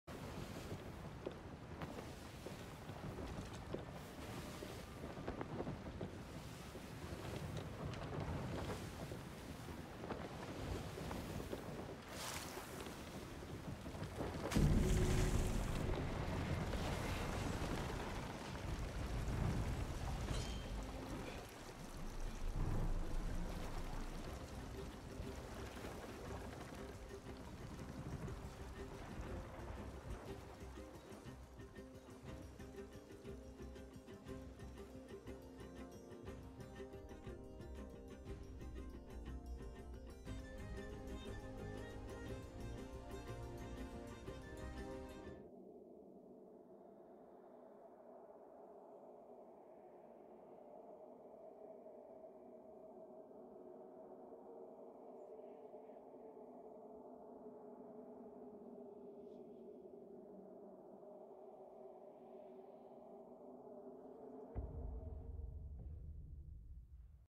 Sinking your ship as the cutscene happens during the Maiden Voyage
I screwed up the timing a bit but it still bugs out